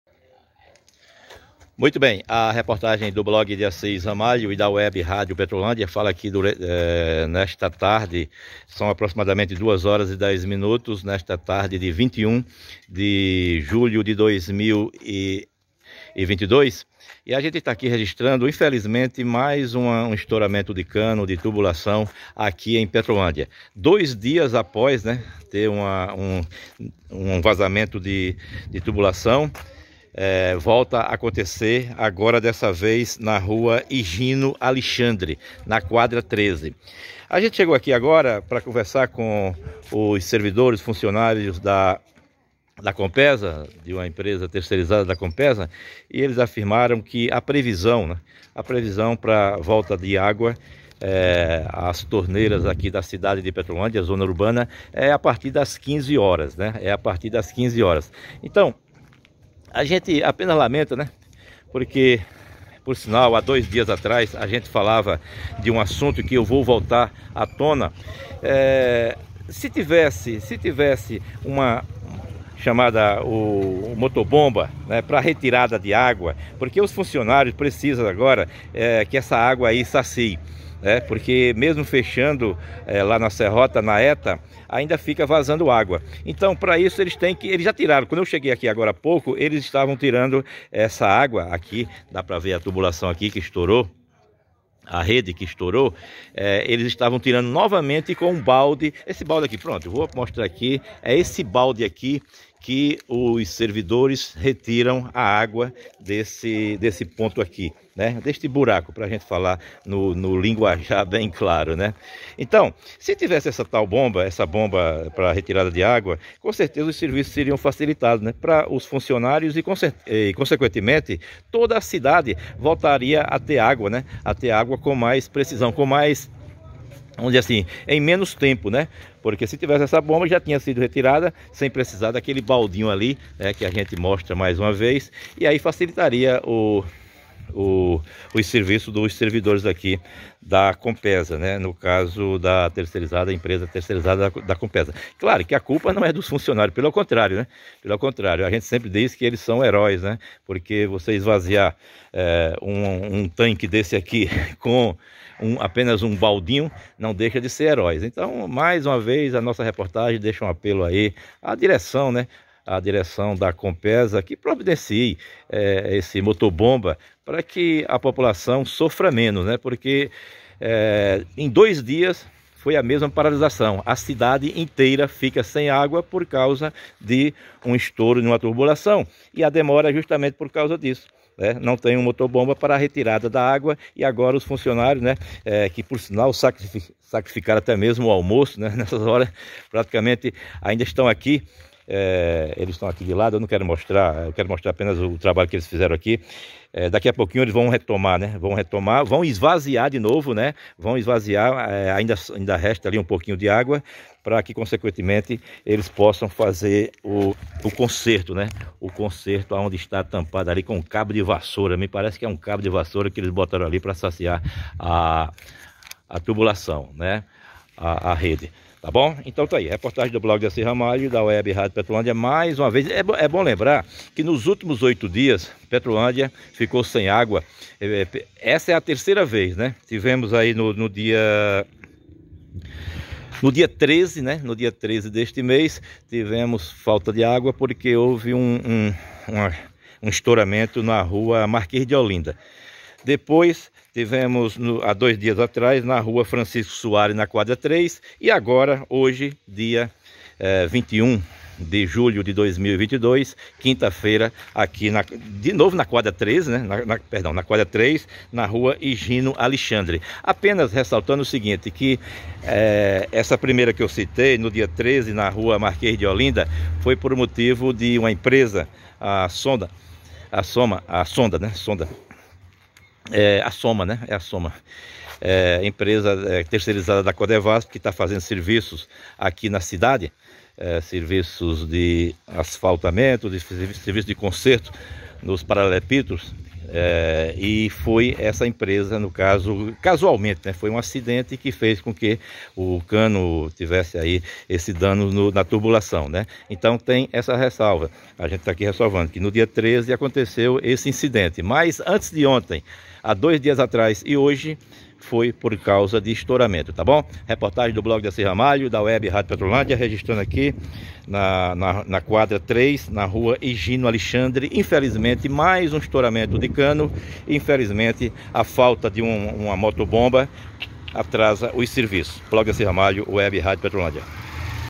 Falta motobomba: Servidores da COMPESA tiram água com balde de plástico para conserto de cano
A agência da Compesa em Petrolândia, informou, nesta quinta-feira (21/07), que a falta de água na cidade foi provocada por um vazamento na tubulação de 60mm na Rua higino Alexandre, na Quadra 03, em frente a Clinica Bangalô [Centro de Atendimento Infantil Especializado].

A reportagem do Blog de Assis Ramalho e Web Rádio Petrolândia esteve no local e registrou os fatos. De acordo com informações colhidas por meio de funcionários da empresa tercerizada da Compesa, os serviços deverão ser concluídos na tarde desta quinta-feira (21), e o restabelecimento de água deverá acontecer a partir das 16 horas.

De modo heroico, os servidores fazem a retirada da água com um simples balde de plástico, como mostra reportagem abaixo.